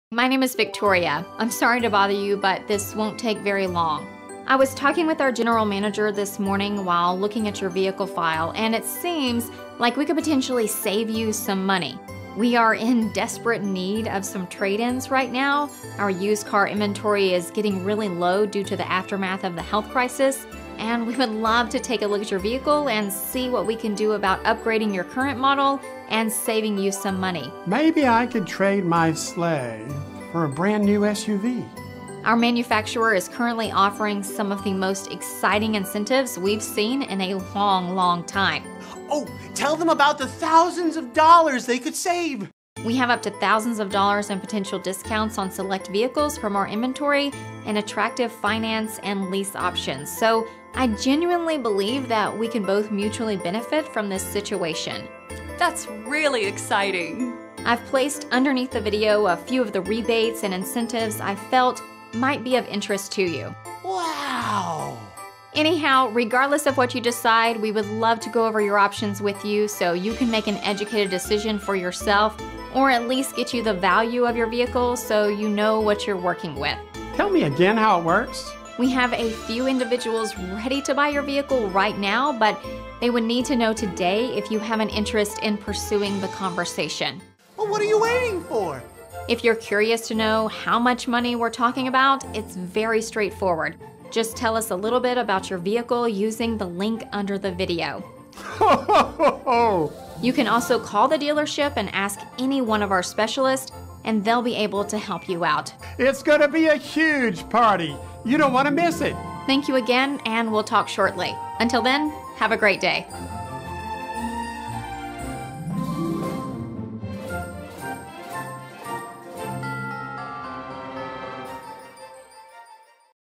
Revenberg GM Holidays 2020: We Need Your Vehicle
Hi, my name is Victoria. I’m sorry to bother you, but this won’t take very long. I was talking with our General Manager this morning while looking at your vehicle file, and it seems like we could potentially save you some money.

We are in desperate need of some trade-ins right now. Our used car inventory is getting really low due to the aftermath of the health crisis, and we would love to take a look at your vehicle and see what we can do about upgrading your current model and saving you some money.

Our manufacturer is currently offering some of the most exciting incentives we’ve seen in a long, long time. We have up to thousands of dollars in potential discounts on select vehicles from our inventory and attractive finance and lease options, so I genuinely believe that we can both mutually benefit from this situation.

I’ve placed, underneath the video, a few of the rebates and incentives I felt might be of interest to you.

Anyhow, regardless of what you decide, we would love to go over your options with you so you can make an educated decision for yourself, or at least get you the value of your vehicle, so you know what you’re working with.

We have a few individuals ready to buy your vehicle right now, but they would need to know today if you have an interest in pursuing the conversation.
If you’re curious to know how much money we’re talking about, it’s very straightforward. Just tell us a little bit about your vehicle using the link under the video.

You can also call the dealership and ask any one of our specialists, and they’ll be able to help you out.

Thank you again, and we’ll talk shortly.

Until then, have a great day.